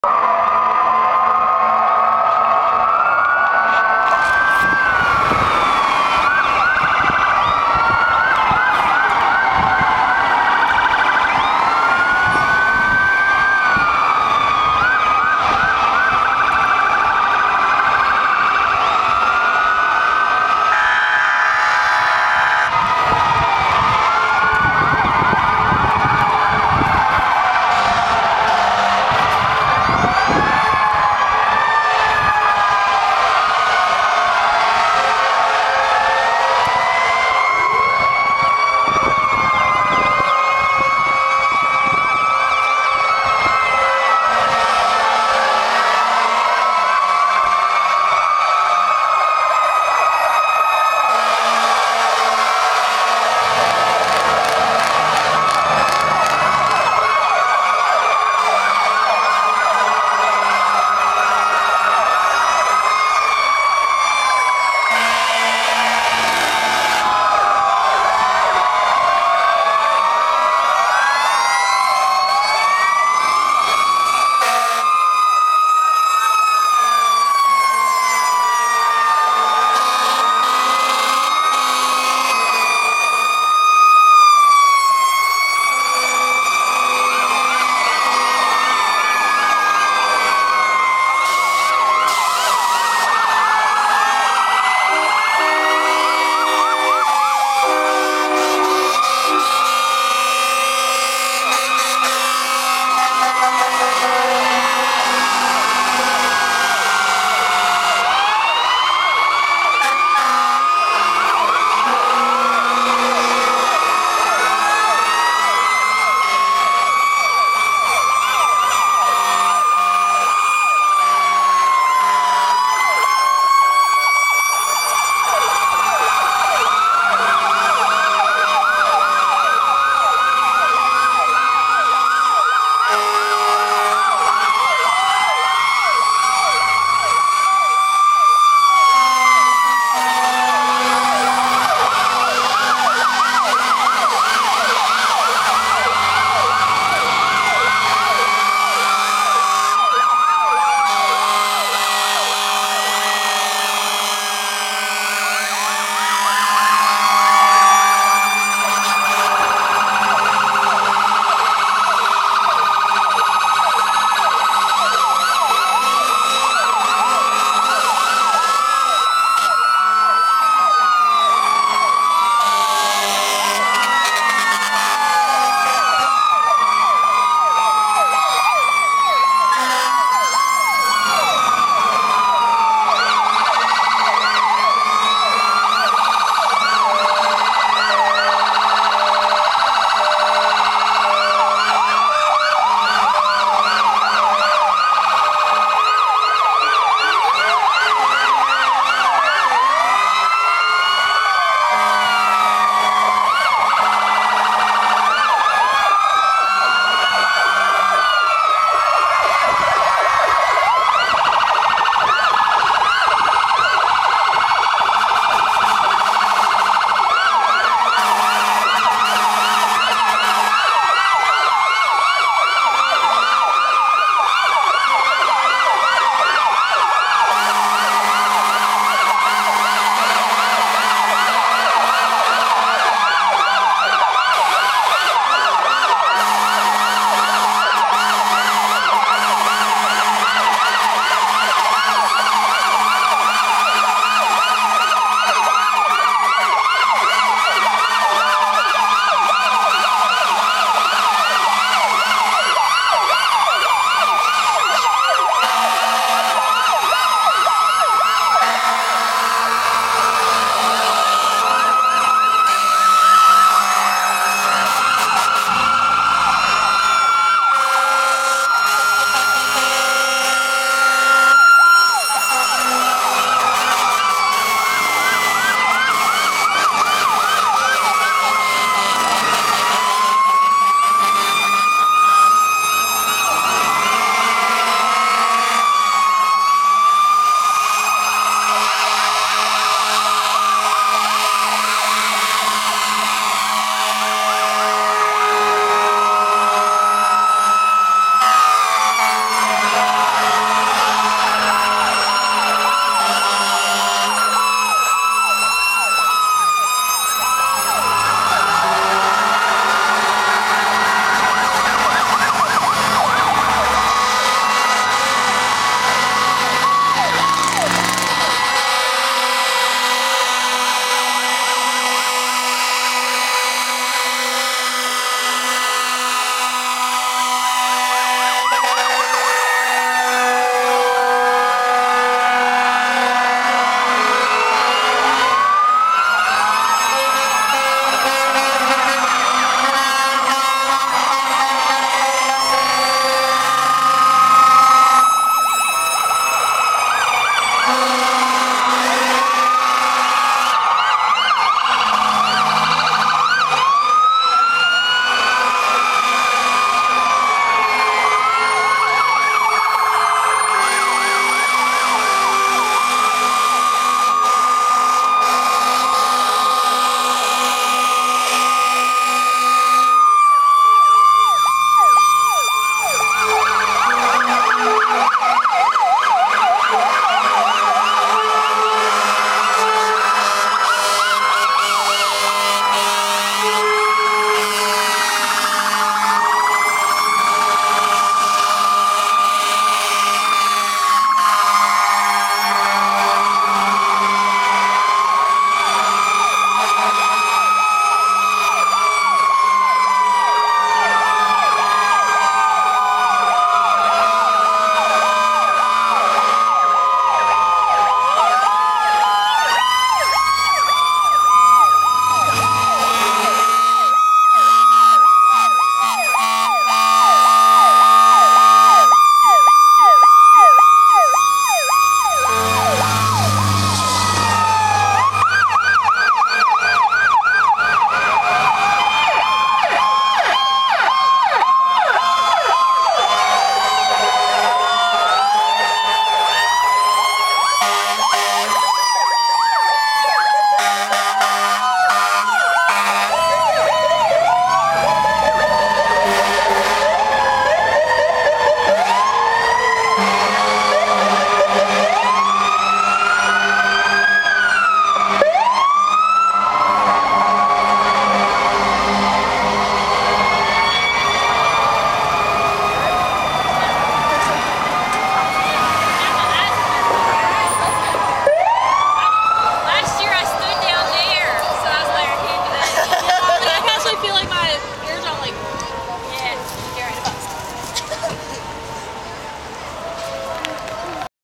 Apple Blossom Festival Firemen's Parade Post-Ceremony Mass Exit 2011
Fire trucks from around the area annually make a big exit after finishing the Apple Blossom Festival Firemen's Parade.

The fun begins around 1:10.